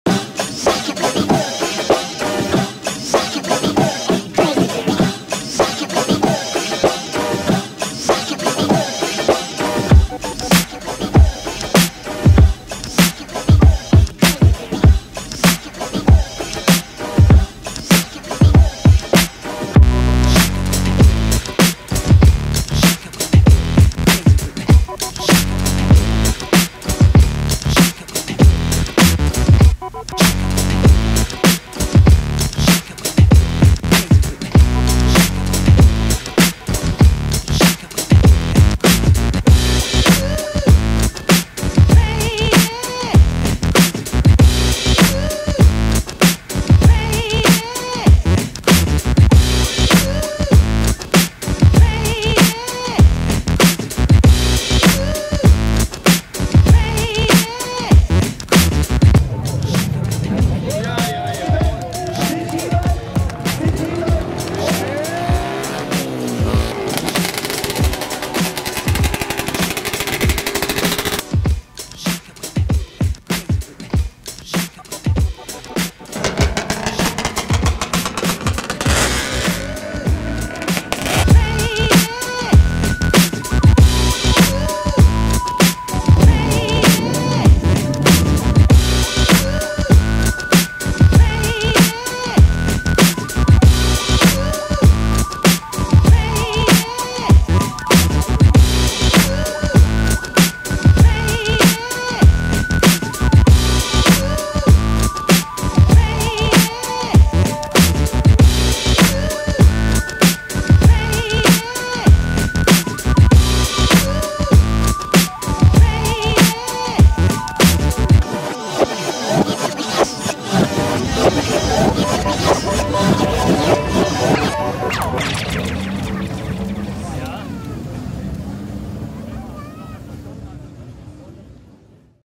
Glemseck 101 - 2017
We didnt plan to go there but we had a bike delivery the same day in that area, so we grabbed the cam. It was too crowed and our time was limited.
So these are some shorts clips of the bikes we saw while running through. Unfortunately we missed the 1/4mile races.